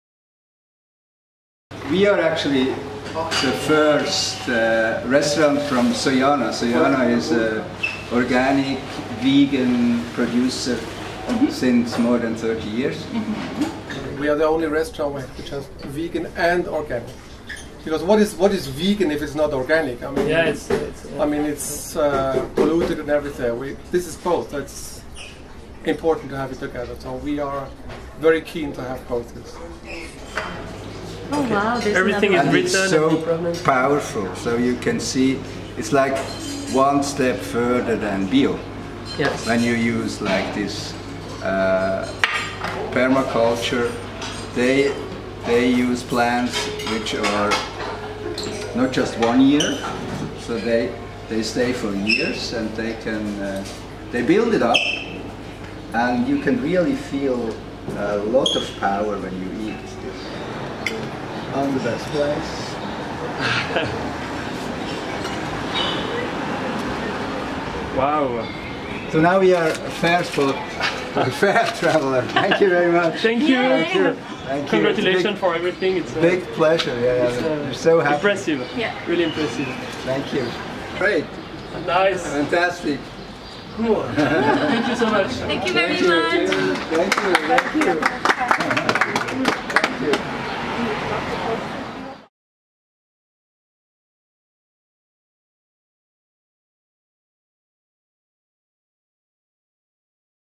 fairpractice vegan organic food and icecream - Vegelateria
The first day of the swissfaircrawl, we were at this wonderful vegan, raw, vegeterian restaurent with innovative approach to cooking. All the ingredients are organic, most of them are produced locally, with only exotic once being imported, and they are so directly from the farmers and small producers.